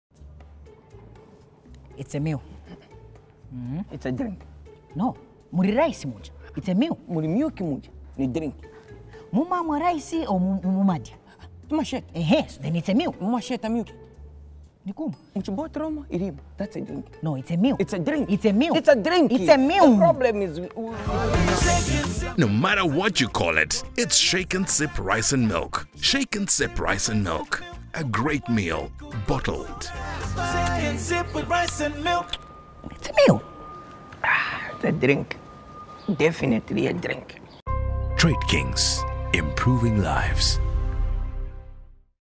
Shake n Sip Rice & Milk The Great Debate
Shake n Sip Rice & Milk Advert Feat Nicholas Raymond Banda & Cosmas Ngandwe Logic
A Bigtree & Trade Kings Product